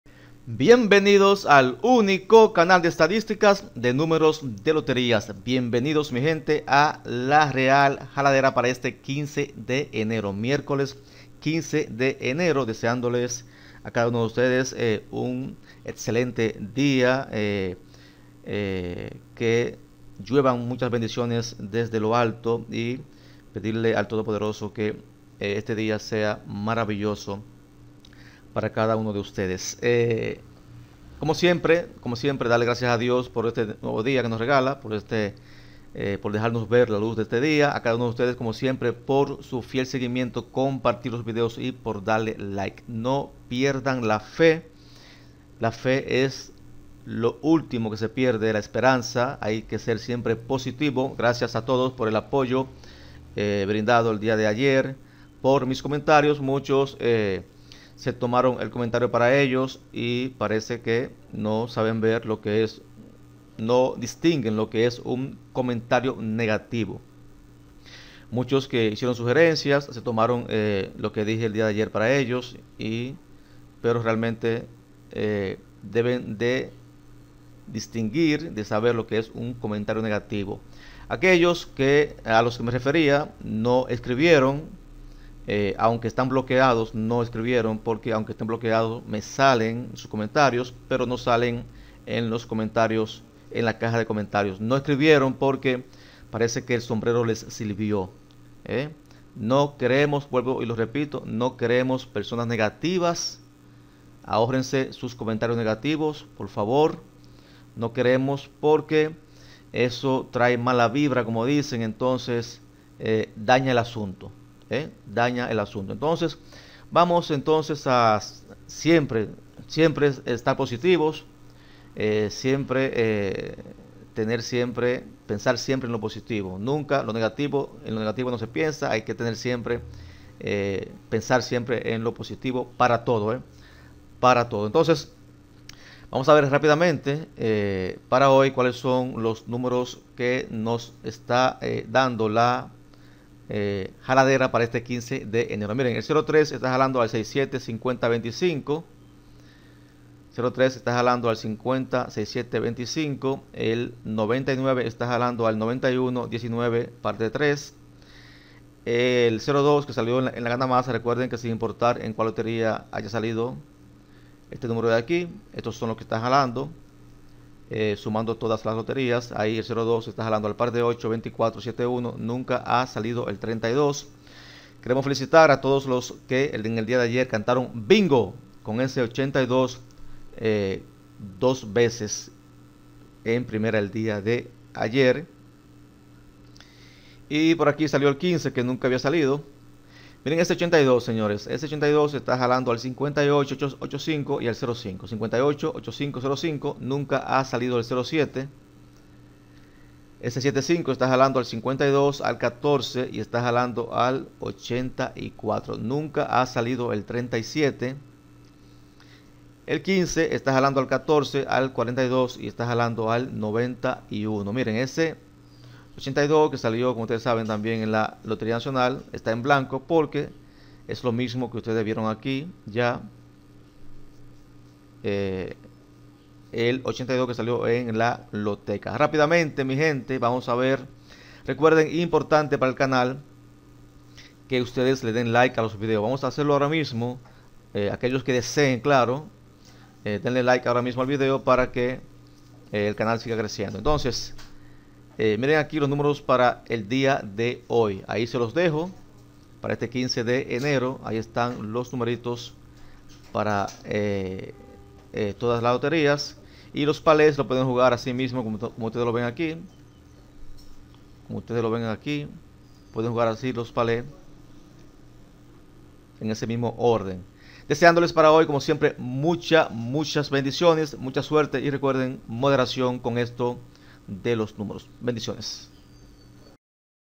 NUMEROS PARA ESTE MIERCOLES 15 Ene -La Real Jaladera de los Numeros-
Aqui te enteraras de:
-Cuales son los numeros con mayores posibilidades para este dia.
-Los pales que nunca han salido y que mas han salido.
-Los números con más de un año que no salen en primera en cada una de las loterías.
-Los números con más dias sin salir en ninguna de las posiciones.
-Que tiempo tenían sin salir y tienen que no salen en las demás loterias, los números que acaban de salir.
-La veces que han salido los numeros un dia como hoy
-Entérese del tiempo que tienen los números sin salir, según sus inicios y terminales, por loterias.
-Entérese de los números con poquísimas y pocas posibilidades de salir en primera x loteria.
-De eso y mucho más, solo en este canal.